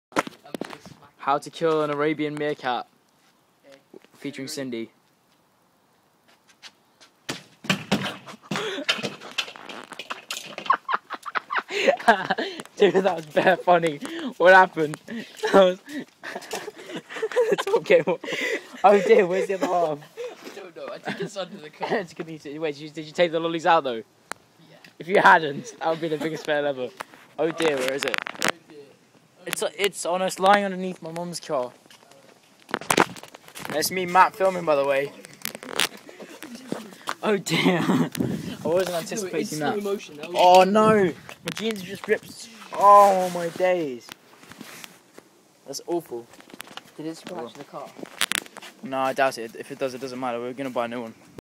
How To: Kill a Toy Meerkat with a Giant Pink Lollipop
Lol my mate killed a meerkat with a giant lollipop